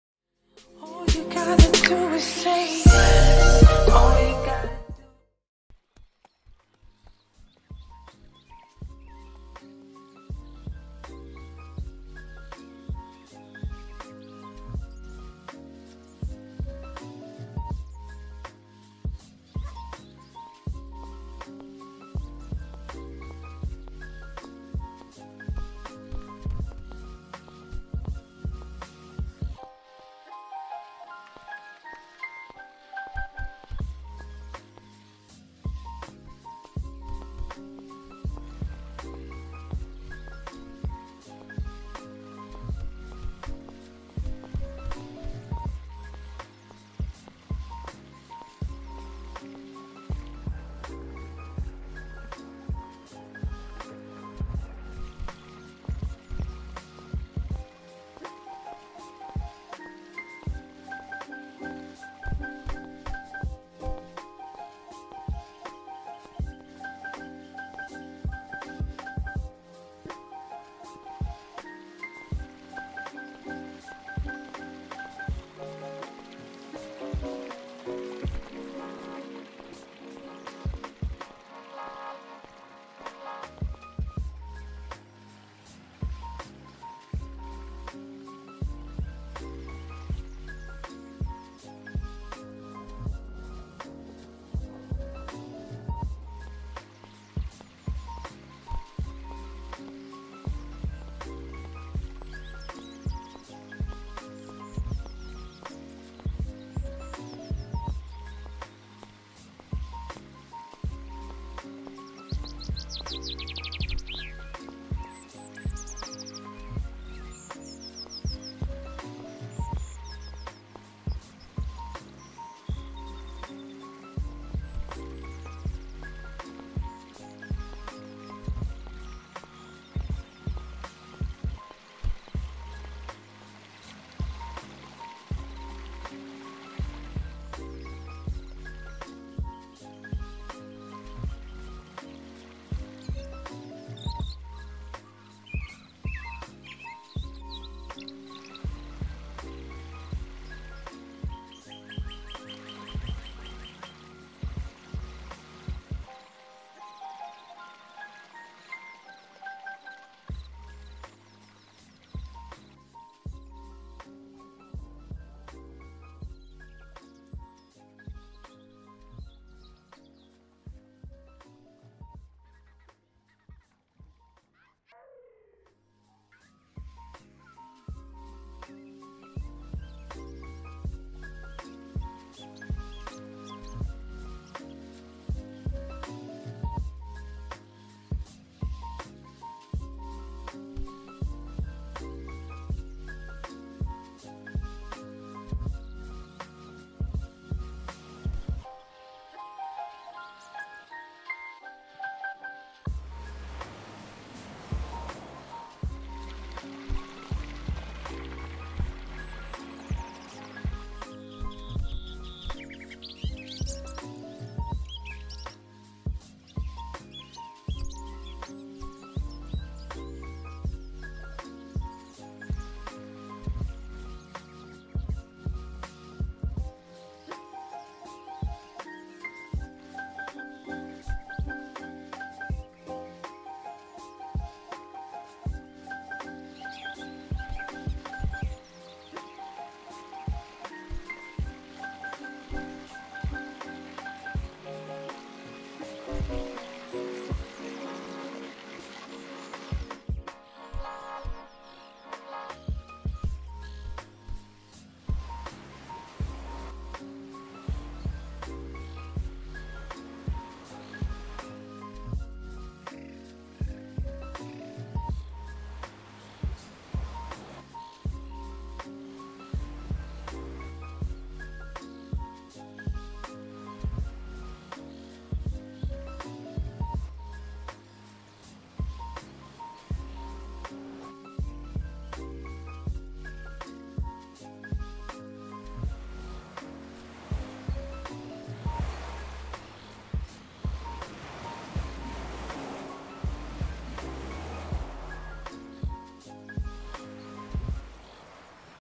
Silent Vlog: Sightseeing
Hey, y'all. We will be going on a beautiful sightseeing tour that's also a silent vlog.